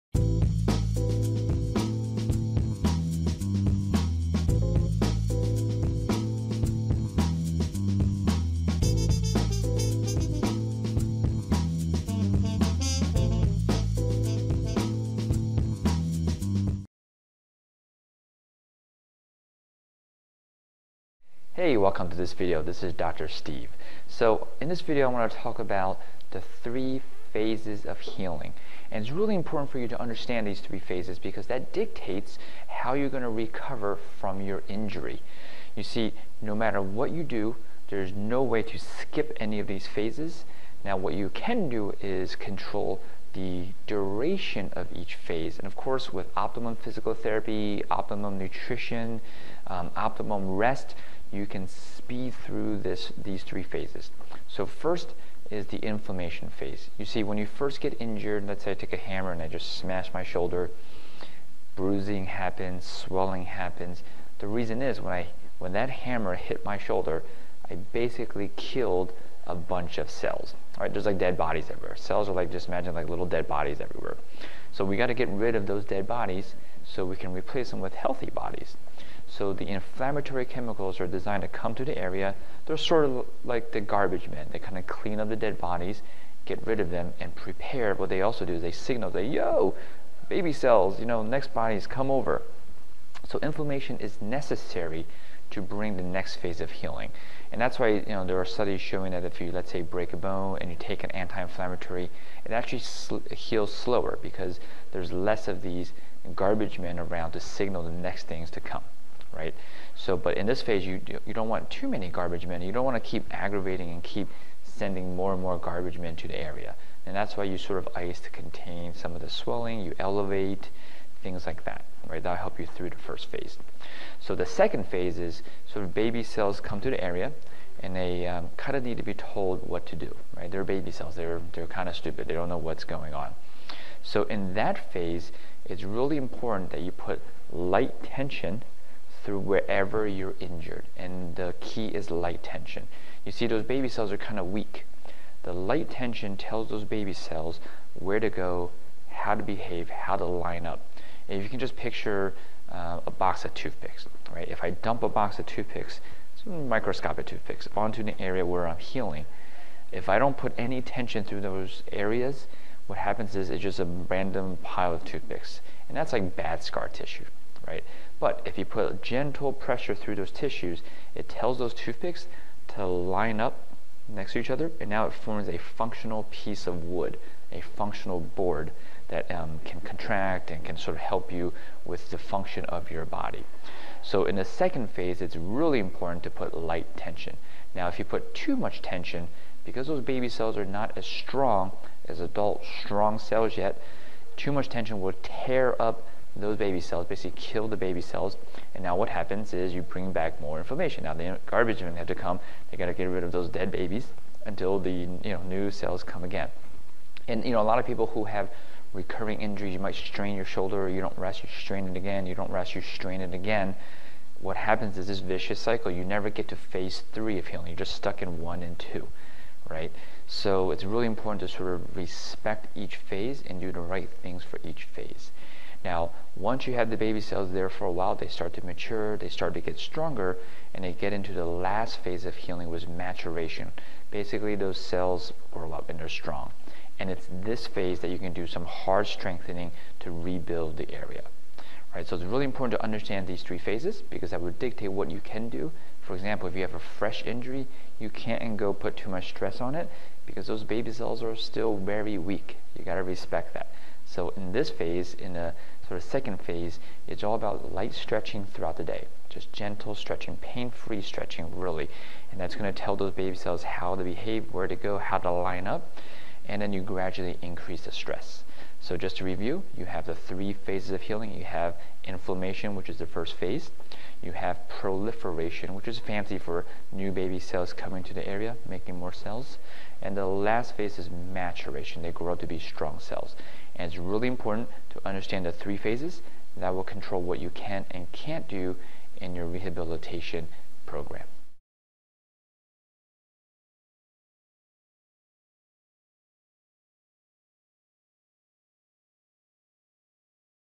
Phases Of Healing Explained-Phases of Wound Healing
0:10.9 you hey welcome to this video this is dr. Steve so in this video I want to talk about the three phases of healing and it's really important for you to understand these to be phases because that dictates how you're going to

0:33.2 recover from your injury you see no matter what you do there's no way to skip any of these phases now what you can do is control the duration of each phase and of course with optimum physical therapy optimum nutrition

0:49.3 optimum rest you can speed through this these three phases so first is the inflammation face you see when you first get injured let's say I take a hammer and I just smash my shoulder bruising
happens swelling happens the reason is

1:04.9 why when that hammer hit my shoulder I basically killed a bunch of cells all right there's like dead bodies everywhere cells would like just imagine like little dead bodies everywhere so we got to get rid of those day bodies so we

1:17.1 can replace them with healthy bodies so the inflammatory chemicals are designed to come to the area there's sort of like the garbageman they kind of clean up the dead bodies get rid of them and prepare what they also do they signal their yo

1:29.4 baby cells you know next bodies come over so inflammation is necessary to bring the next phase of healing and that's why you know there are studies showing that a few let's say break a bone and you take an anti-inflammatory

1:41.5 it actually heal slower because there's less of these garbagemen around to signal the next things to come right so but in this phase you do you don't want too many garbageman you don't want to keep aggravating and keep sending more

1:55.2 and more garbage man to the area and that's why you sort of I used to contain some of the swelling you elevate things like that right now help you through the first phase so the second phase is so baby cells come to the area

2:08.1 I kind of need to be told what to do right there baby cells there they're kind of stupid they don't know what's going on so in that phase it's really important that you put light tension through wherever you're injured and the

2:21.4 key is light tension you see those baby cells are kind of weak the light tension tells those baby cells where to go how to behave how to line up if you can just picture a box of toothpicks alright if I dump a box of toothpicks microscopic to

2:37.3 fix on to the area where I'm healing if I don't put any tension through those areas what happens is it just a random pile of toothpicks and that's like bad scar tissue right but if you put a gentle pressure through those tissues it

2:51.1 tells those toothpicks to line up next to each other and now it forms a functional piece of wood a functional board that M can contract and can sort of help you with the function of your body so in the second phase it's really

3:05.8 important to put light tension now if you put too much tension because those baby cells are not as strong as adults strong sales yet too much tension will tear up those babies celibacy kill the baby cells and now what happens is you

3:19.8 bring back more information now they are vigilant have to come they gotta get rid of those dead babies until the you know new cells come again and you know a lot of people who have recurring injuries you might strain your shoulder you don't

3:31.1 rest restraint and again you don't rest your strain it again what happens is this vicious cycle you never get to face three of healing you're just stuck in one and two right so it's really important to sort of respect each phase

3:43.2 and do the right things for each phase now once you have the baby cells there for a while they start to mature they started to get stronger and they get into the last phase of healing was maturation basically those cells or

3:55.5 loving or strong and it's this phase that you can do some hard strengthening to rebuild the area all right so it's really important to understand these three phases because I would dictate what you can do for

4:06.1 example if you have a fresh injury you can't go put too much stress on it because those babies cells are still very weak you got to respect that so in this phase in a sort of second phase it's all about light stretching

4:18.1 throughout the day just gentle stretching pain-free stretching really and that's going to tell those babe yourselves how to behave where to go how to line up and then you gradually increase the stress so just to review